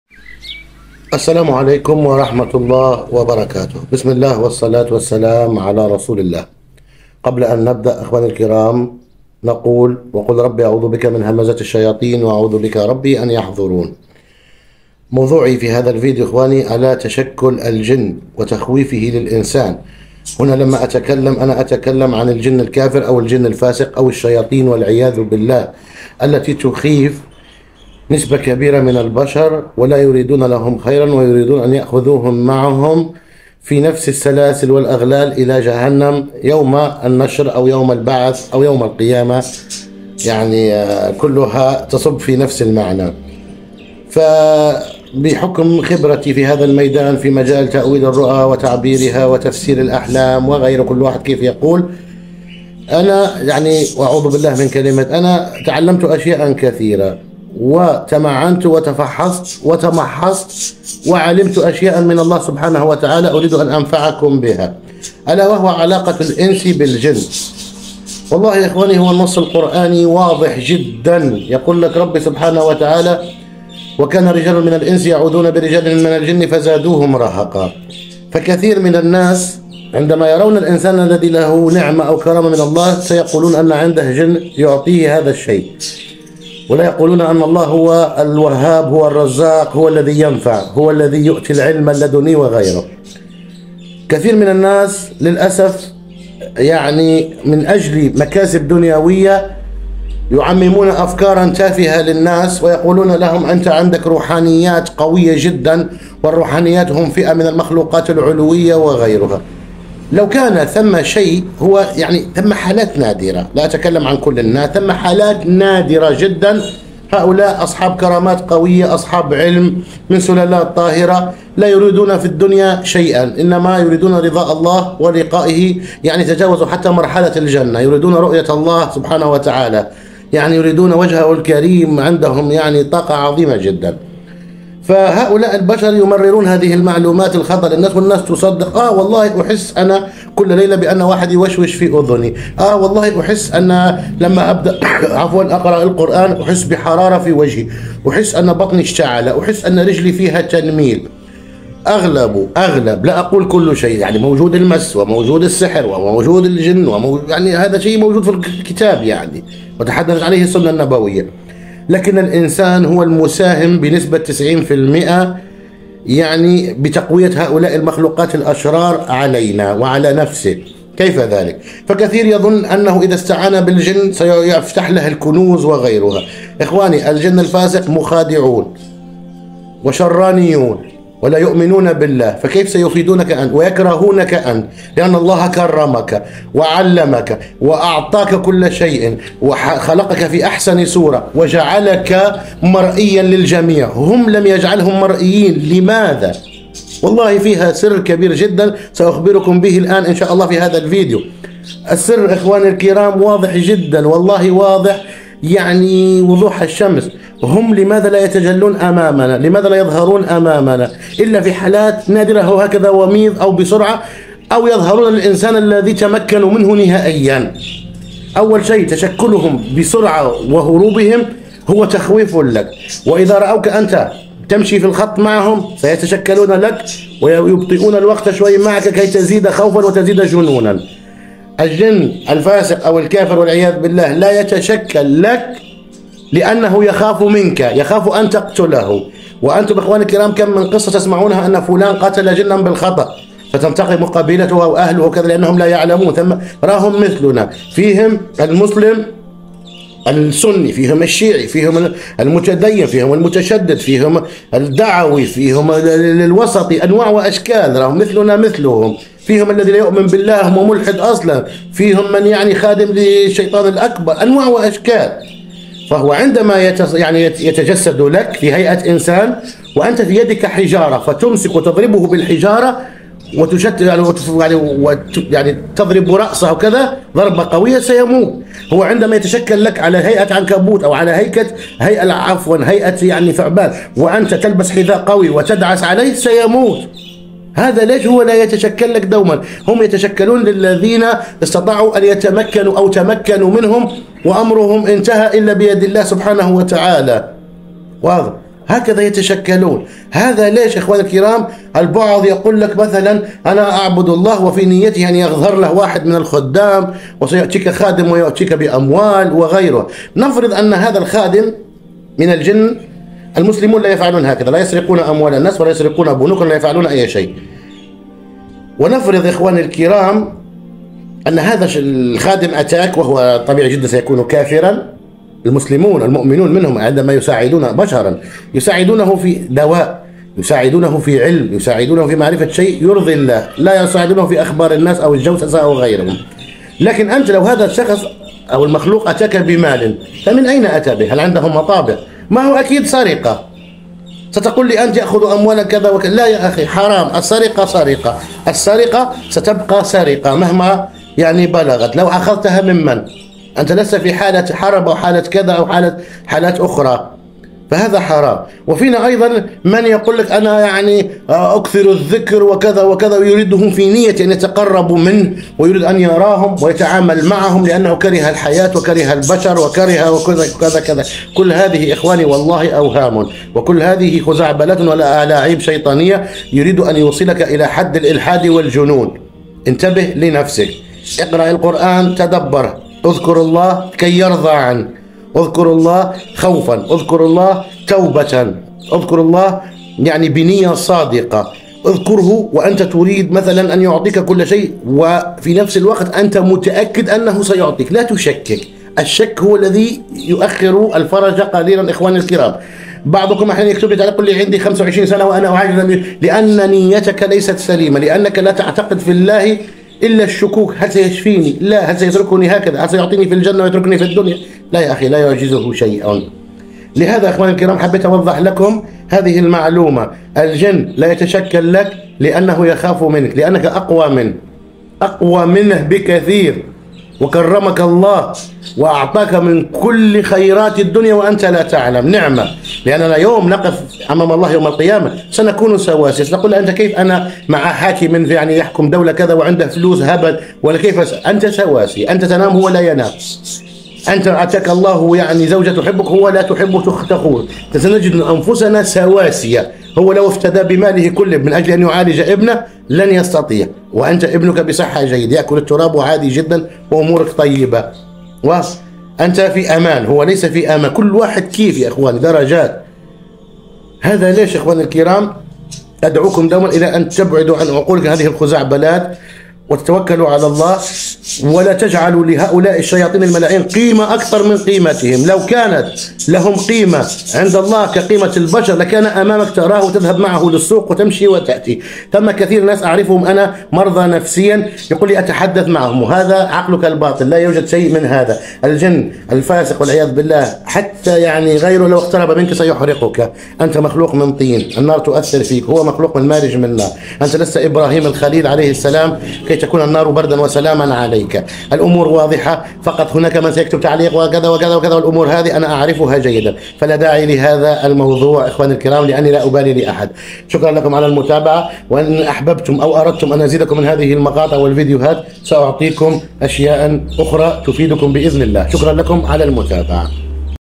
لماذا لا يتجسد الجن أمامنا ونراهم معلومات ستصدمك🫣🫣🫣
الجن (جمع كلمة “جان”) هم مخلوقات خارقة للطبيعة، يُعتقد أنها تمتلك عقولًا وفهمًا.

يُطلق عليهم أيضًا اسم “جني” (للذكور) و"جنية" (للإناث).

يُعتقد أنهم يستترون ولا يُرى عادةً، ولذلك سُمّوا بهذا الاسم.

يُشار إلى وجود الجن في القرآن الكريم، وهم مخلوقات مع الإنسان